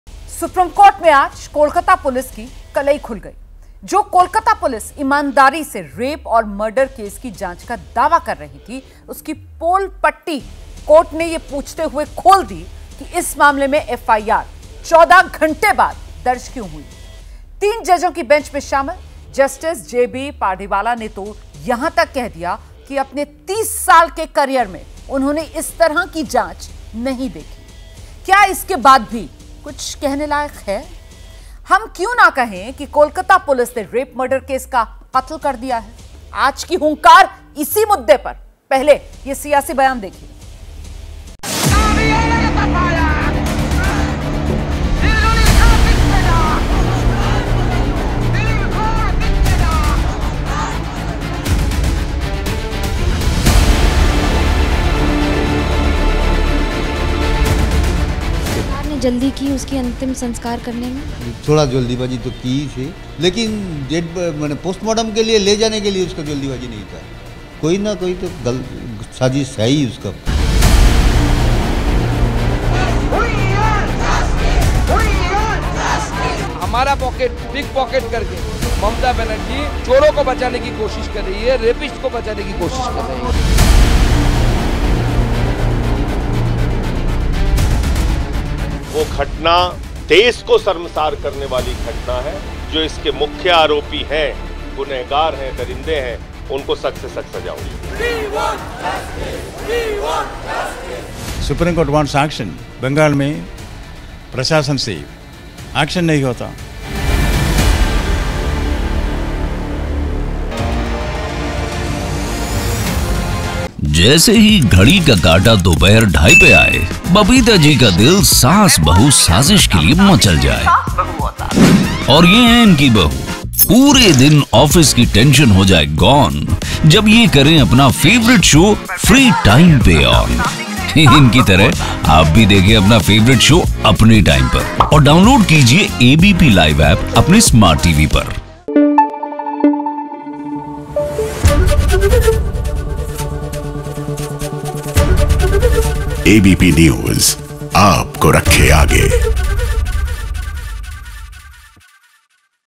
Kolkata Doctor News: सीबीआई की क्या तैयारी...डॉक्टर घोष की होगी गिरफ्तारी ? | ABP News | Breaking
सुप्रीम कोर्ट में आज कोलकाता पुलिस की कलई खुल गई...जो कोलकाता पुलिस ईमानदारी से रेप और मर्डर केस की जांच का दावा कर रही थी, उसकी पोल पट्टी कोर्ट ने ये पूछते हुए खोल दी कि इस मामले में FIR 14 घंटे के बाद दर्ज क्यों हुई...3 जजों की बेंच में शामिल जस्टिस जे बी पारदीवाला ने तो ये तक कह दिया कि अपने 30 साल के करियर में उन्होंने इस तरह की जांच नहीं देखी...क्या इसके बाद भी कुछ कहने लायक है...हम क्यों नहीं कहें कि कोलकाता पुलिस ने रेप-मर्डर केस का क़त्ल कर दिया..